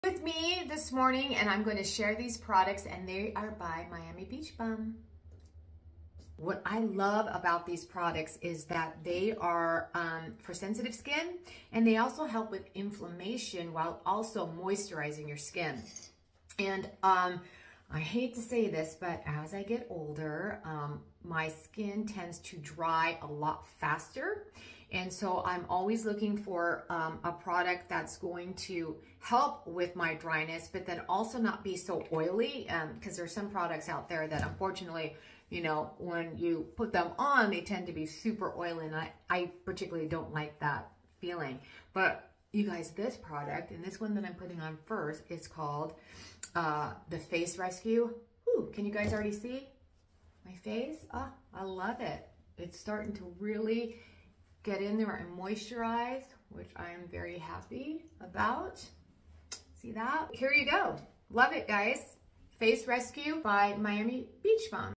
Real Reviews: Morning Routine with Sports Illustrated Rookie Dr. Nina Cash
Get ready with Dr Nina Cash, iconic Sports Illustrated Rookie.

At age 57, Nina won the 2023 Swim Search open casting call, making her SI Swimsuit debut in the 2024 edition. Now, she shares her morning skincare routine, featuring Miami Beach Bum's Face Rescue Cream. Watch to see her review of our hydrating facial moisturizer made for sensitive and dry skin. 💧

Paradise for your skin and hair.